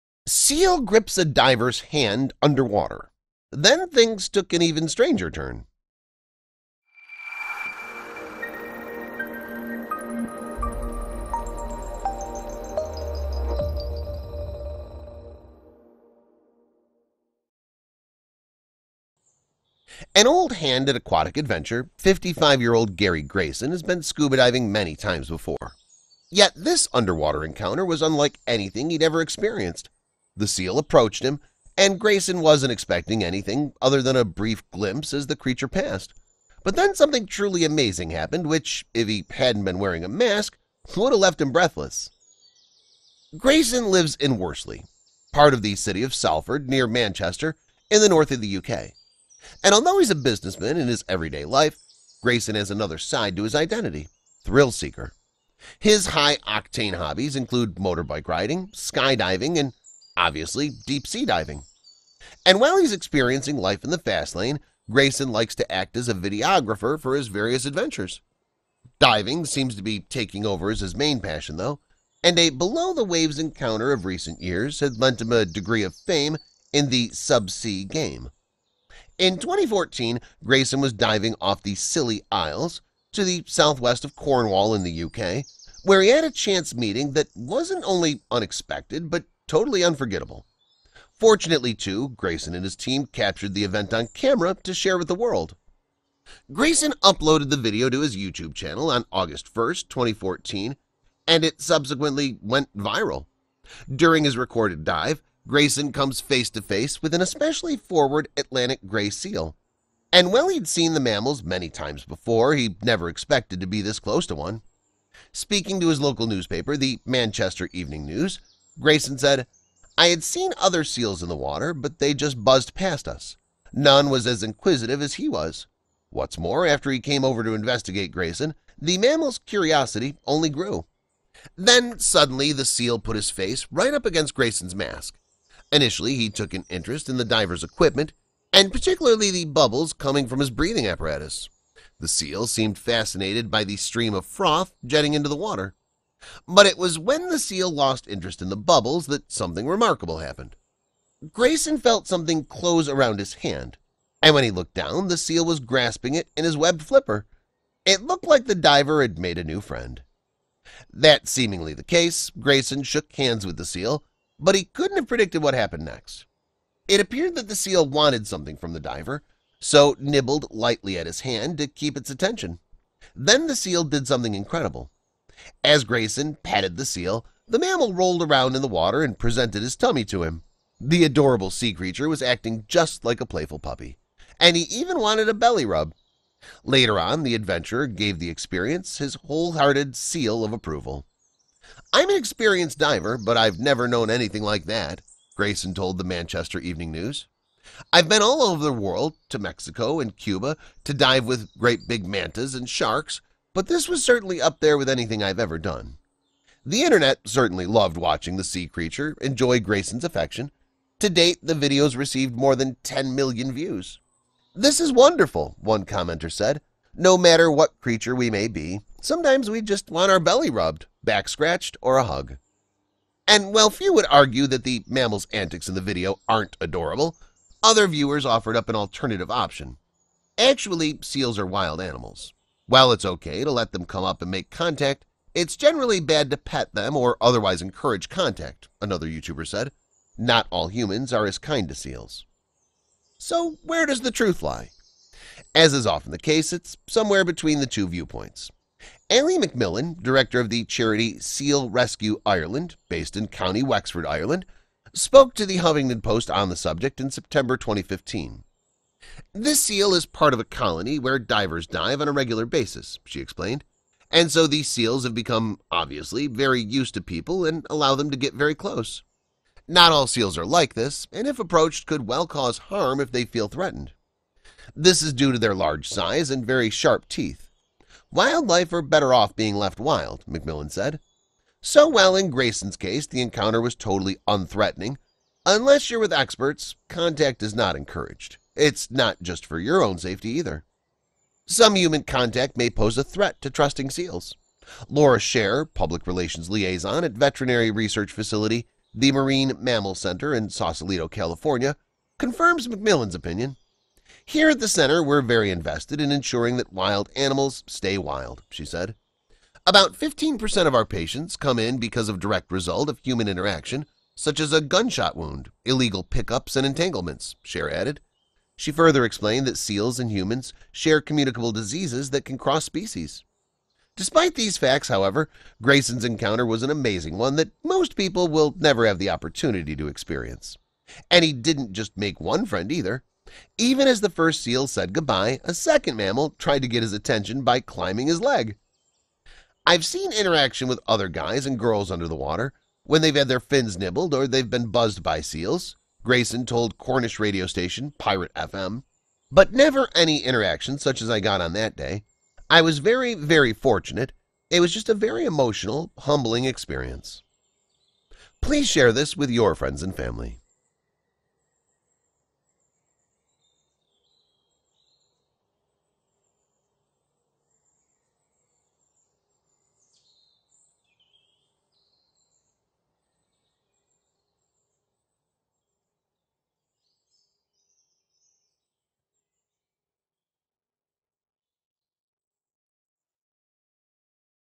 Seal Grips A Diver’s Hand Underwater, Then Things Took An Even Stranger Turn
An old hand at aquatic adventure, 55-year-old Gary Grayson has been scuba diving many times before – yet this underwater encounter was unlike anything he had ever experienced. The seal approached him, and Grayson wasn’t expecting anything other than a brief glimpse as the creature passed. But then something truly amazing happened which – if he hadn’t been wearing a mask – would have left him breathless.

Grayson the thrill-seeker

Grayson lives in Worsley, part of the city of Salford, near Manchester in the north of the U.K. And although he is a businessman in his everyday life, Grayson has another side to his identity: thrill-seeker. His high-octane hobbies include motorbike riding, skydiving and – obviously – deep-sea diving.